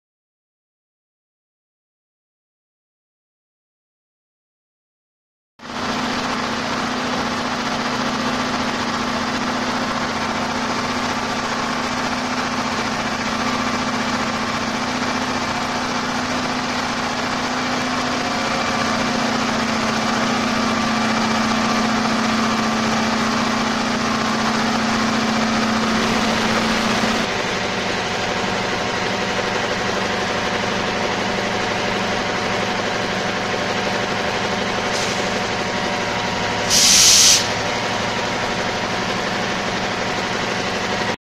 LOCOMOTORA SW1504 EN TALLER CHIHUAHUA
Locomotora SW1504 con el numero 1504  en prueba de carga en taller Chihuahua.
En el video se observa a la locomotora arrojando chispas por los escapes, no es una falla, esas locomotoras juntan aceite en los múltiples y cuando se les aplica toda la potencia por algún tiempo prolongado tienden a arrojar esas chispas.